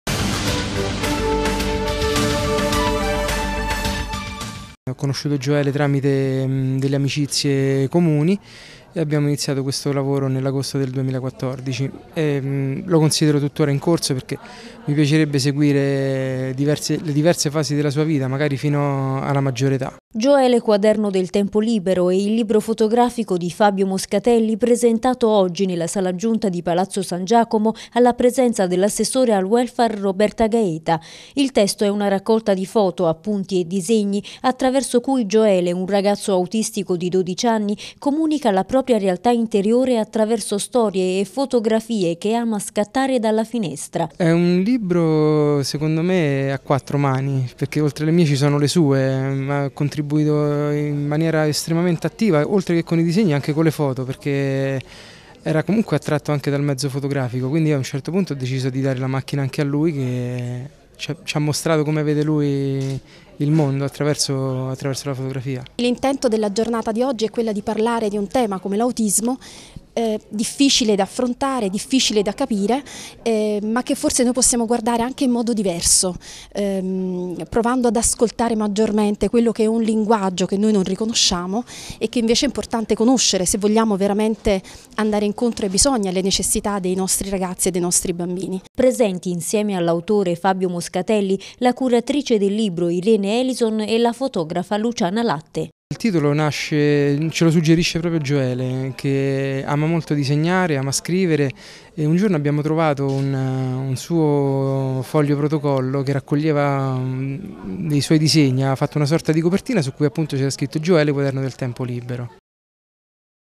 Quaderno del tempo libero, il libro fotografico su Gioele
Presentato in Sala Giunta il libro fotografico di Fabio Moscatelli 'Gioele quaderno del tempo libero'. E' una raccolta di foto, appunti e disegni che raccontano la storia di un ragazzo autistico con la passione per la fotografia.